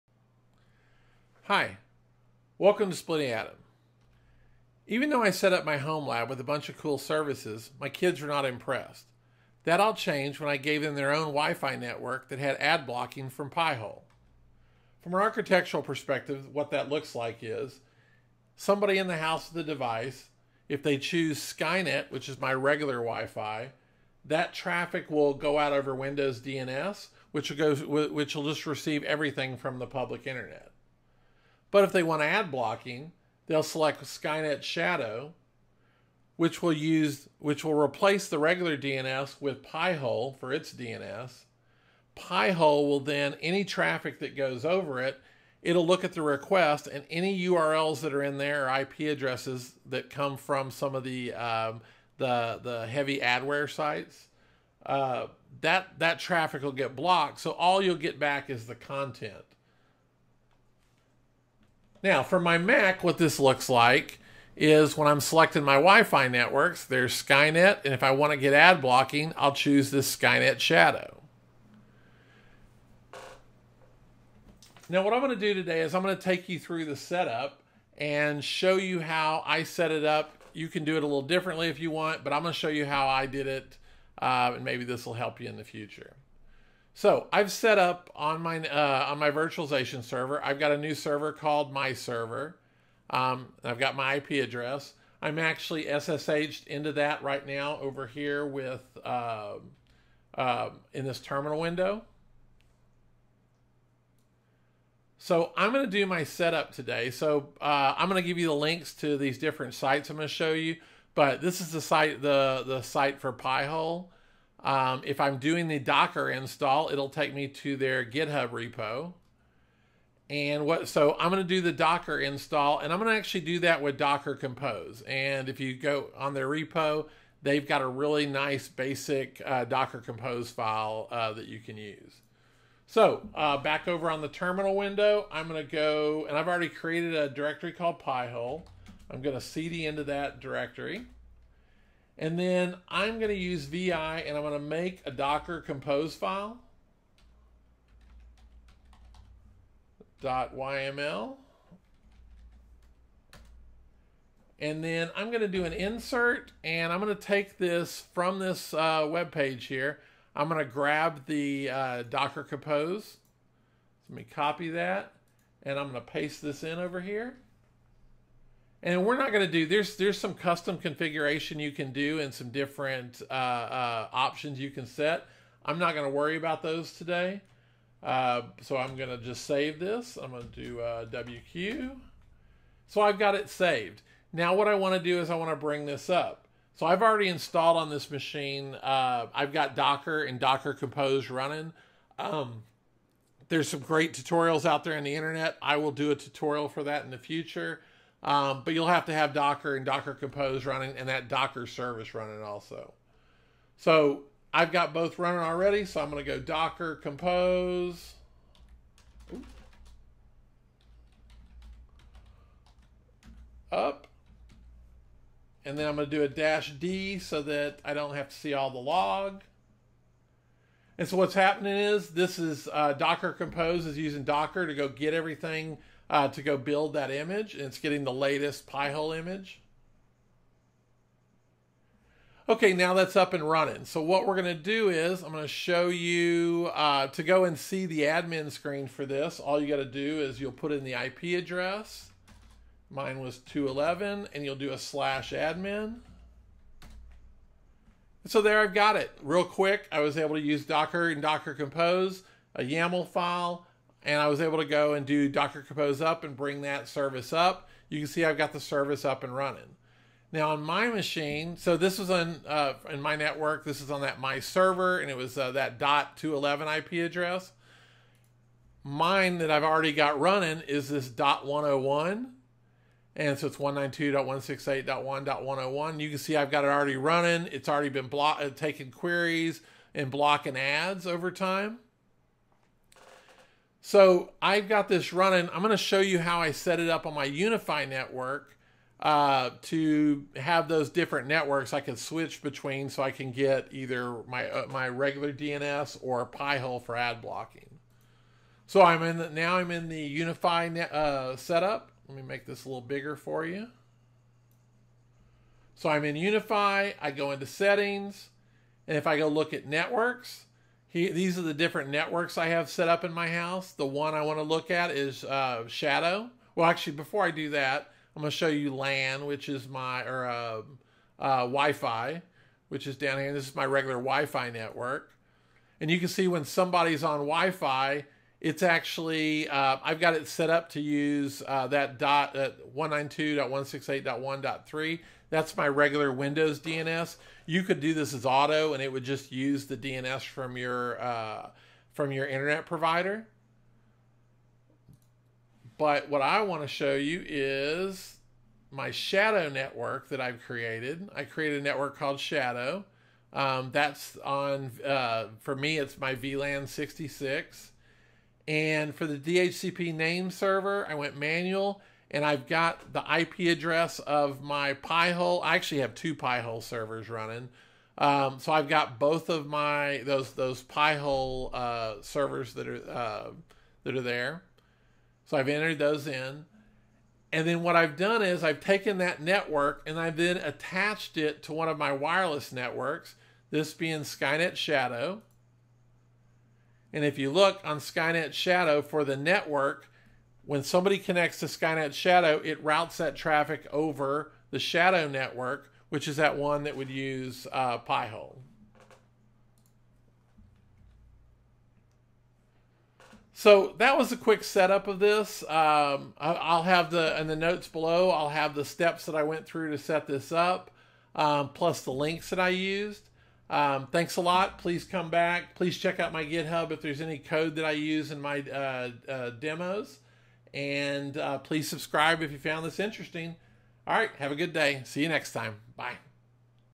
HomeLab 101 - How I made my HomeLab cool for my kids!
Even though I set up my HomeLab with a bunch of cool services,  My kids were not impressed.  That all changed when I gave them their own wifi network that had Ad Blocking with Pi-hole.

This video will take you through the setup for Pi-Hole and how I implement an open network and a network with Ad Blocking from Pi-Hole.